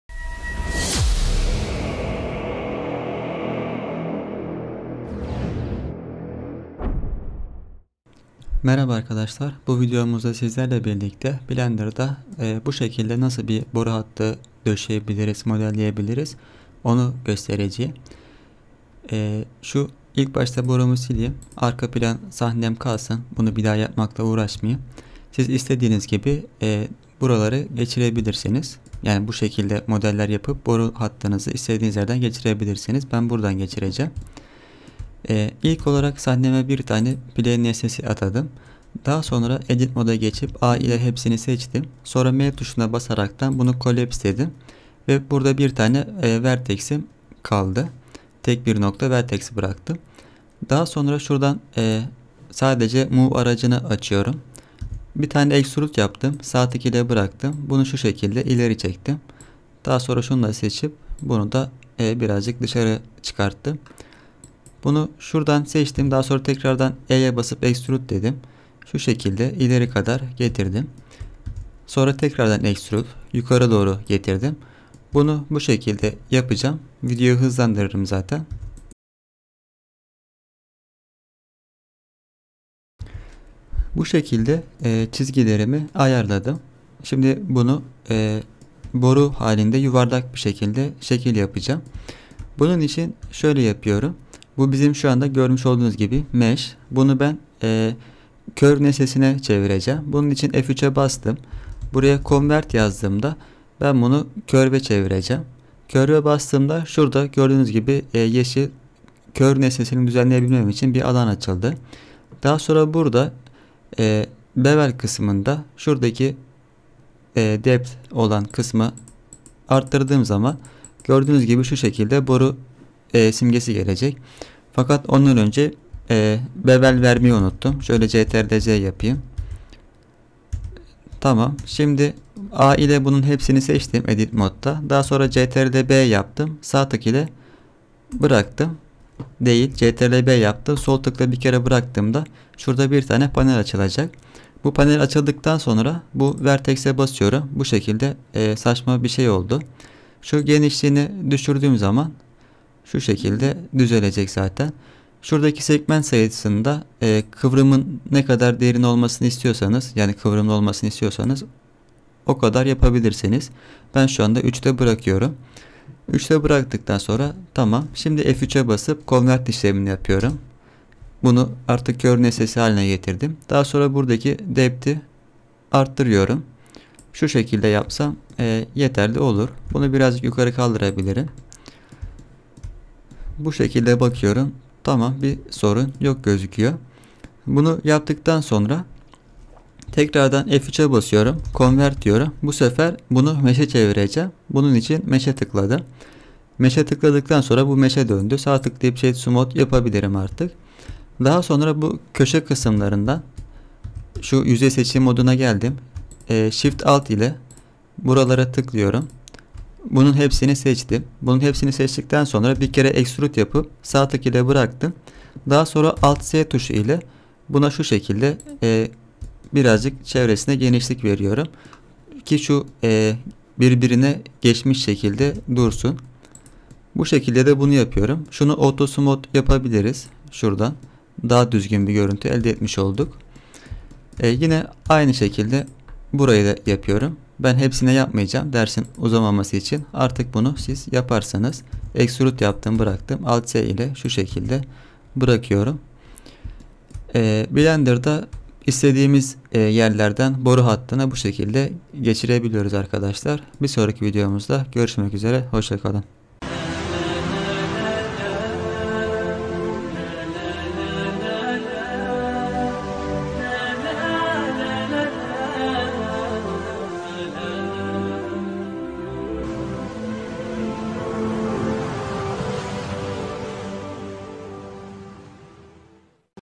Boru Hattı Modelleme(Pipeline Modeling) - Blender Eğitim
Bu videoda blender ile Boru Hattı(Pipeline Modeling) modelledik.

İçerik:
0:00 Giriş
0:10 Tanıtım
0:35 Boru Hattı Modelleme(Pipeline Modeling)

Sorularınız için:
● Discord soru-cevap kanalı